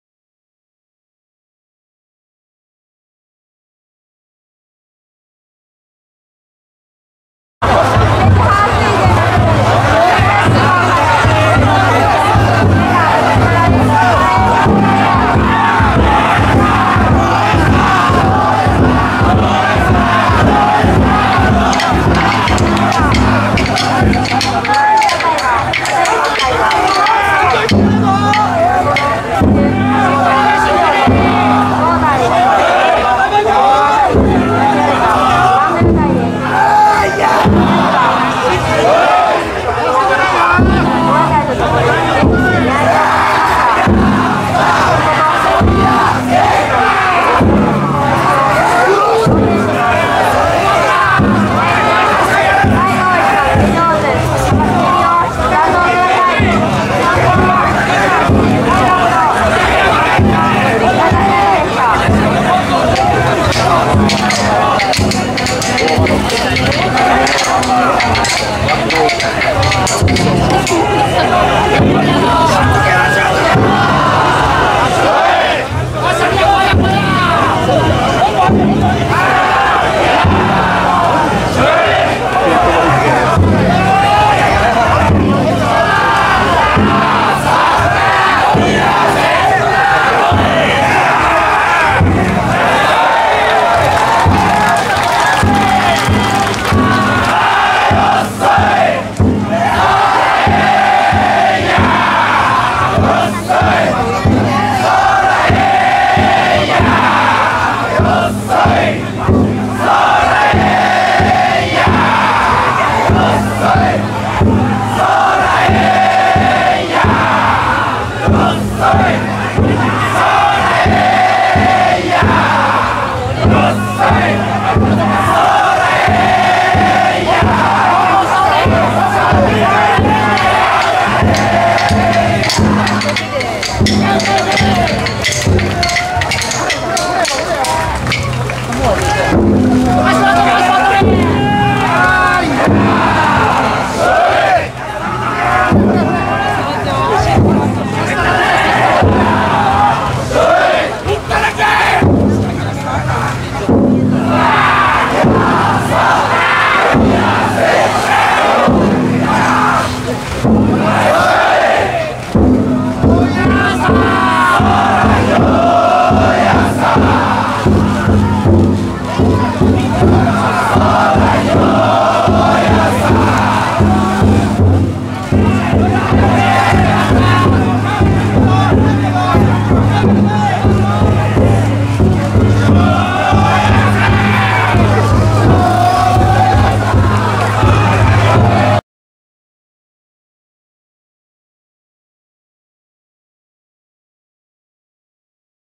恵美酒宮天満神社本宮 玉地宮入（平成２９年１０月９日）
姫路市飾磨区　恵美酒宮天満神社本宮
玉地屋台の宮入です。